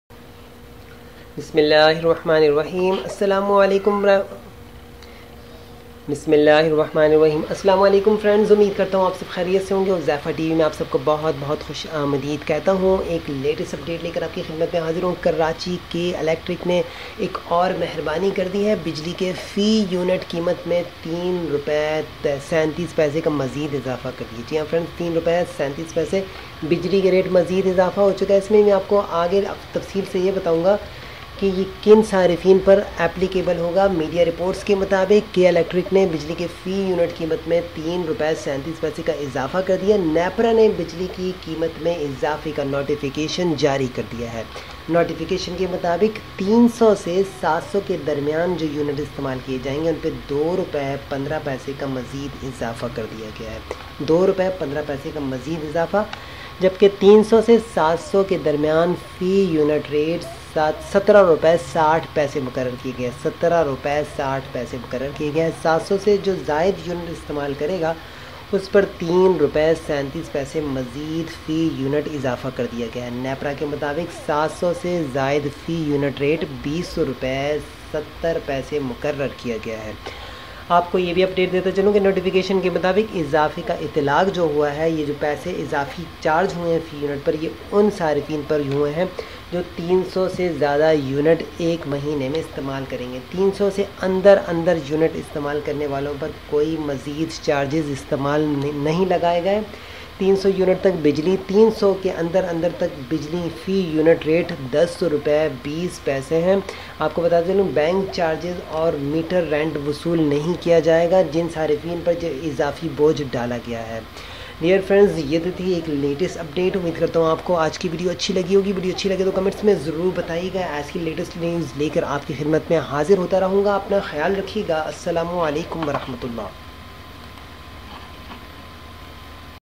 Bijli k Rate ma Mazeed izafa \ K Electric has increased unit rate
Breaking News K Electric has increased Rs 3.8 per unit.

Bijli mazeed  mehngi ho gai.